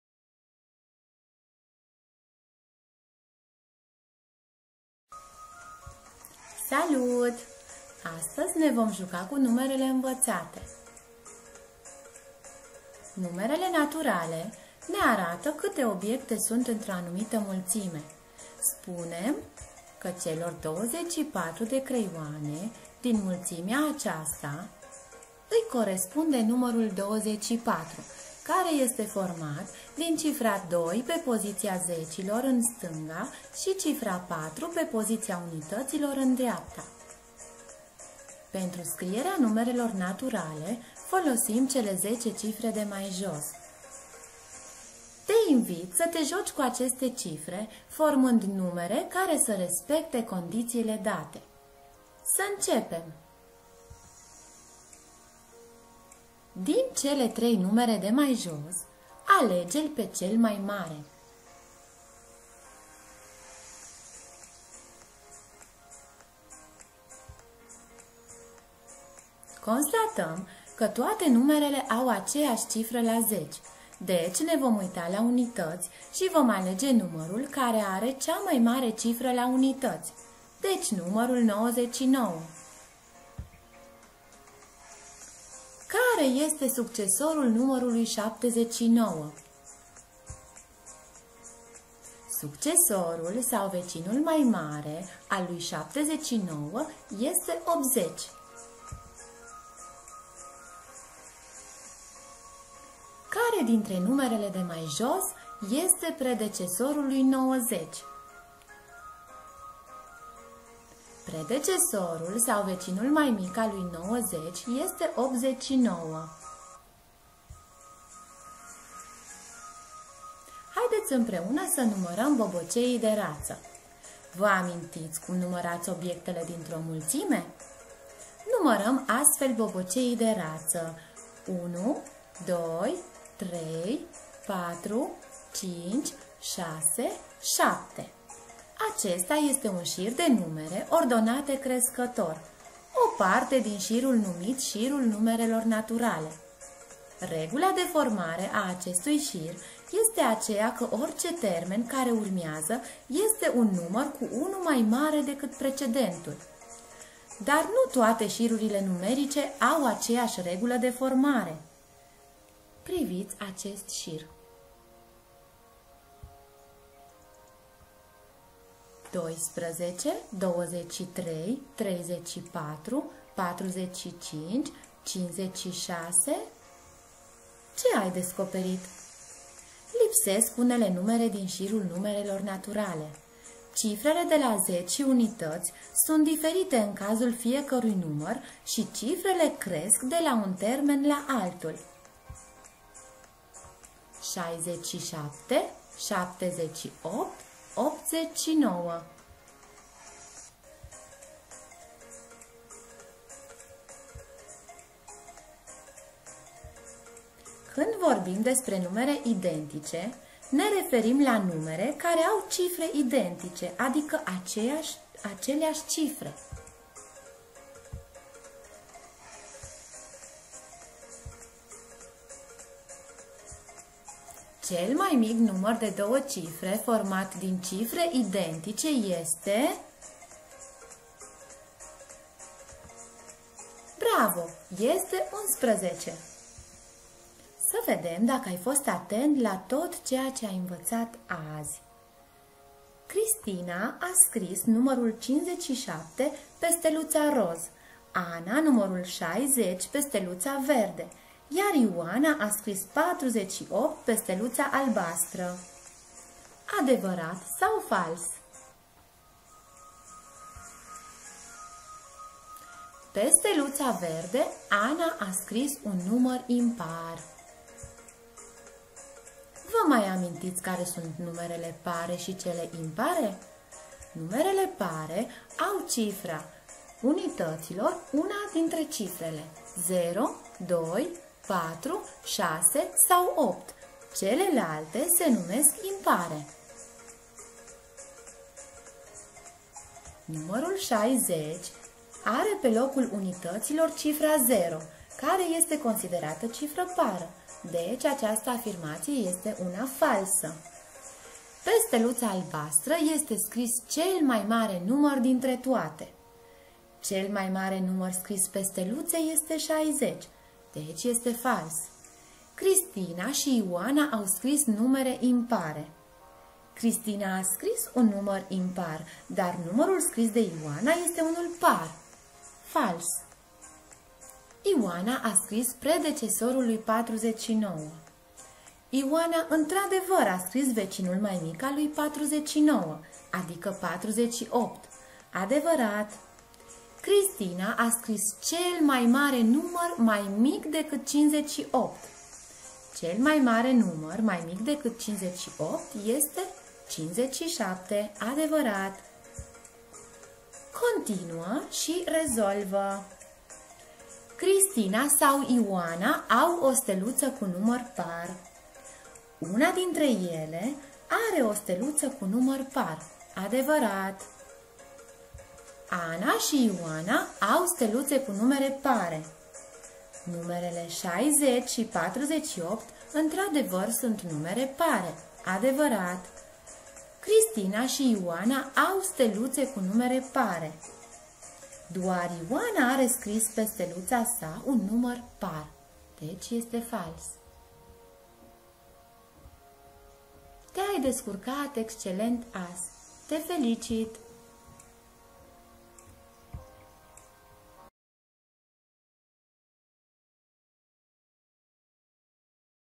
Găsește numărul!
Resursa cuprinde:
- identificarea unui număr respectând anumite condiții: cel mai mare număr mai mic decât 90, cel mai mic număr scris cu două cifre identice;
- completarea unui șir de numere;
- Adevărat sau Fals: identificarea numărului mai mic/mai mare, predecesorul numerelor, numere pare/ impare.

Autor: Gabriela Krisan